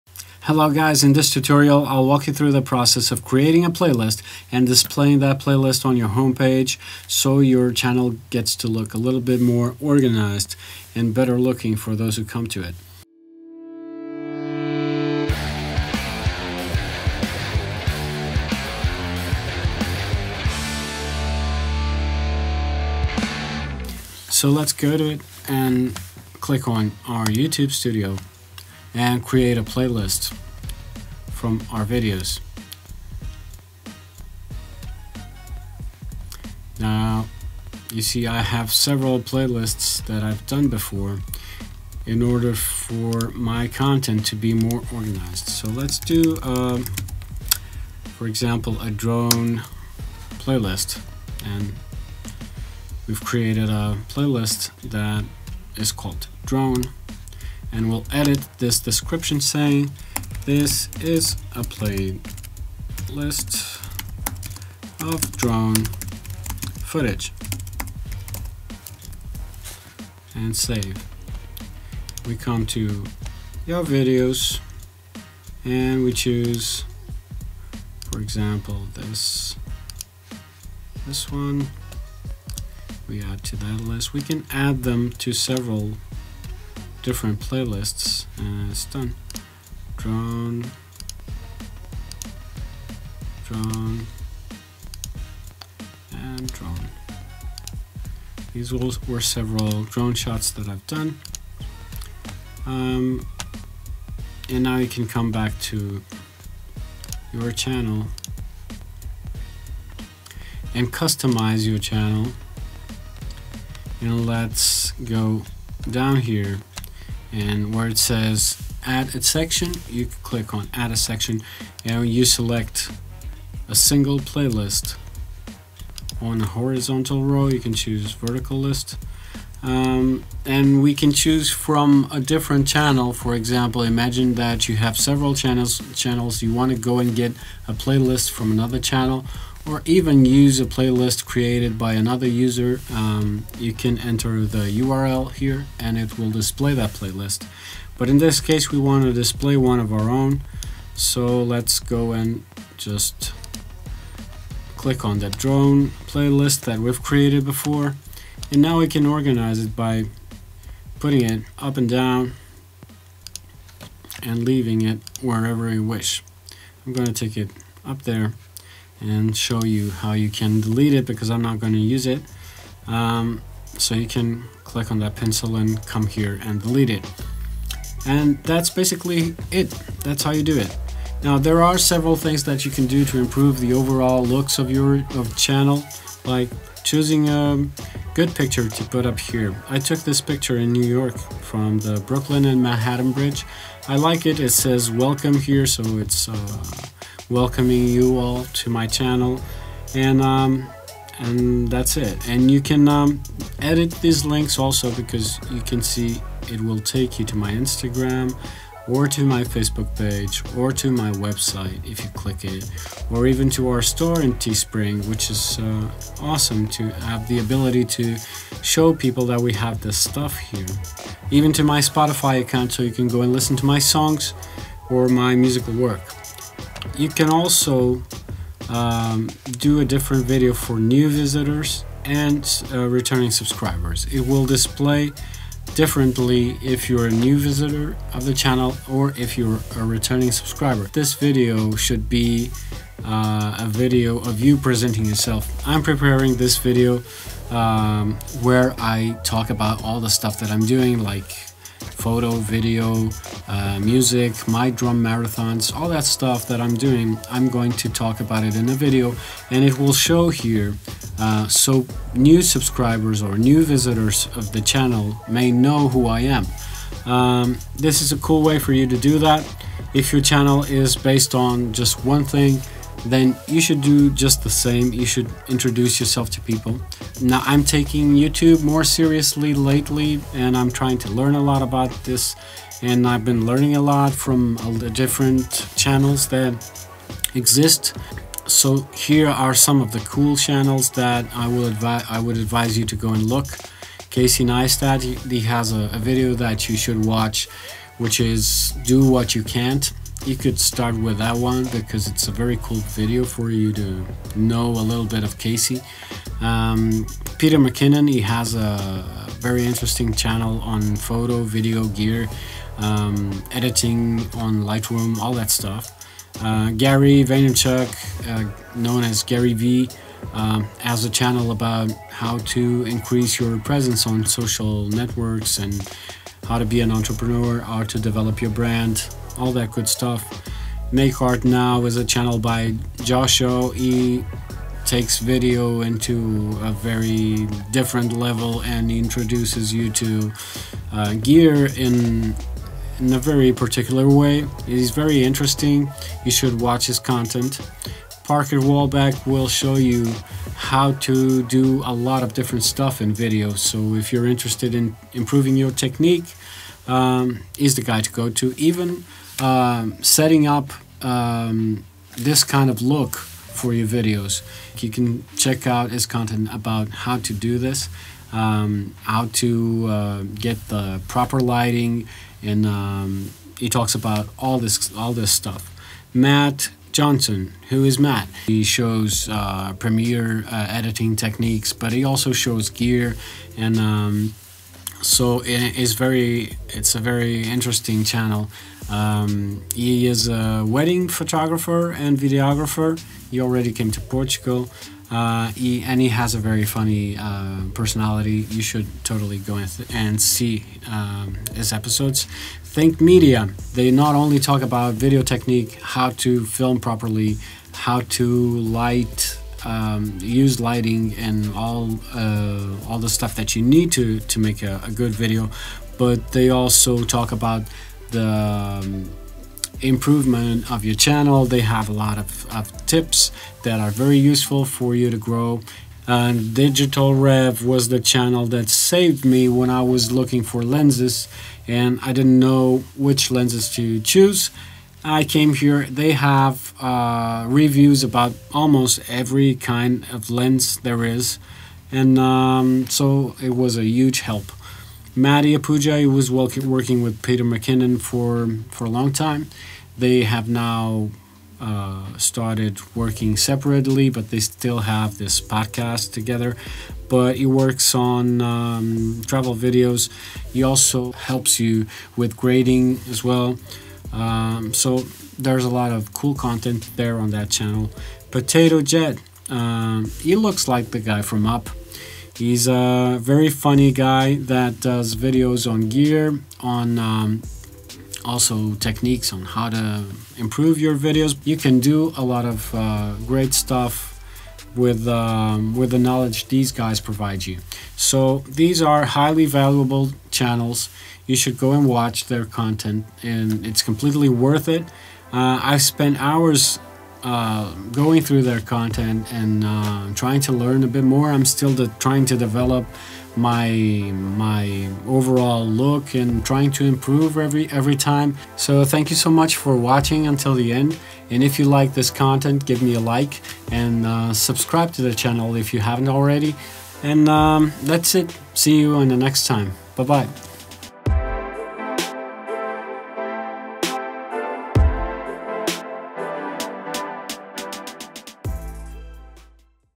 How to organize a youtube channel with playlists [How to display playlists on your homepage]
In this short tutorial I walk you through the process of creating a playlist on your channel homepage and display the several playlists on your home page. Lately, I have been having the need to organize my videos better and I've taken this step, after doing it some of my friends kept on asking me hot to do it, so here it is. I hope it is helpful to you if you haven't done it on your channel yet. I have some more requests to do some other tutorials.

Did you like this tutorial? Would you like me to do any specific one? The best to you all.